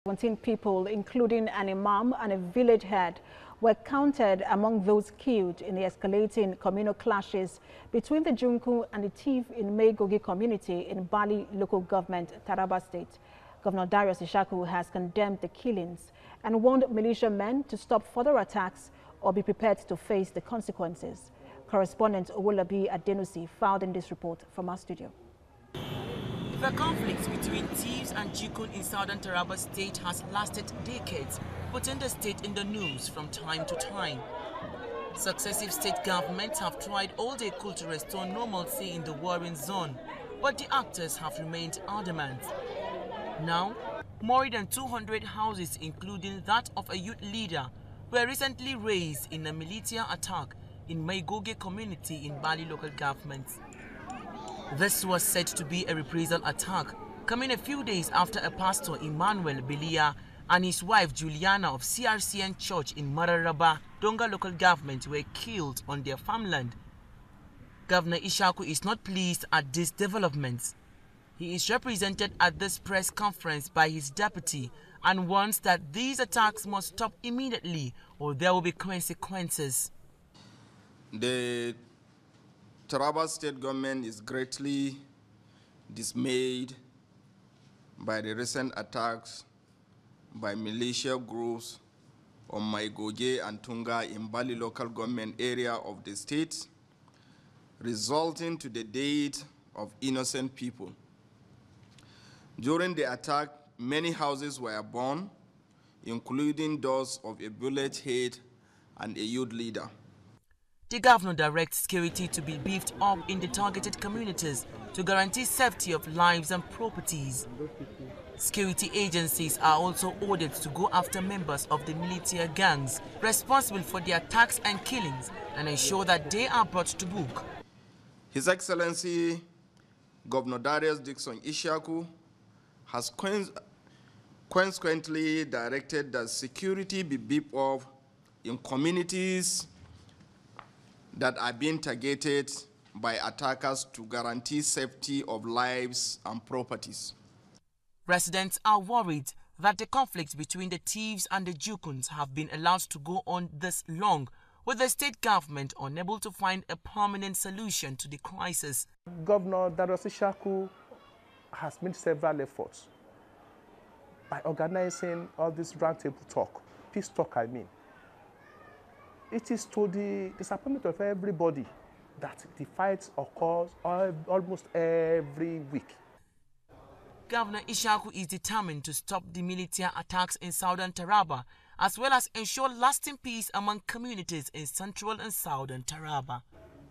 Militia kills 17 persons in escalating communal clashes between Jukun and Tiv
17 people including an imam and a village head were counted among those killed in the escalating communal clashes between the Jukun and the Tiv in Maigoge community in Bali local government, Taraba State.

 Governor Darius Ishaku has condemned the killings and warned militiamen to stop further attacks or be prepared to face the consequences.